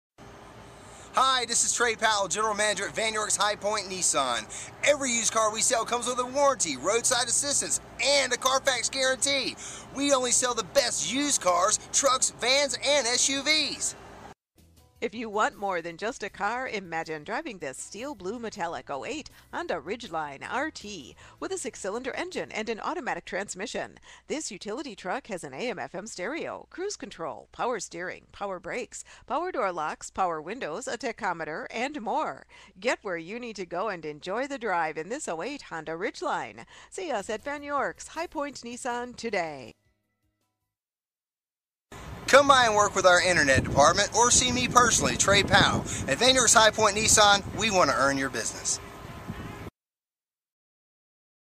Honda Ridgeline, Vann York's High Point Nissan- High
Honda Ridgeline, Vann York's High Point Nissan Vann York's High Point Nissan knows you want more than just a car. you have a purpose for your vehicle. you will love this Steel Blue Metallic 2008 Honda Ridgeline, equipped with a 6 Cyl. engine and an automatic transmission with only 25,877 miles. enjoy this utility truck with features like AM/FM Stereo Radio, Cruise Control, Power Steering, Power Brakes, Power Door Locks, Power Windows, Tachometer, and much more. Get where you need to go, enjoy the drive, and have peace of mind in this 2008 Honda Ridgeline. see us at Vann York's High Point Nissan today!
miles-  25,877
vin- 2HJYK16218H525597

1810 S. Main ST.
High Point, North Carolina 27261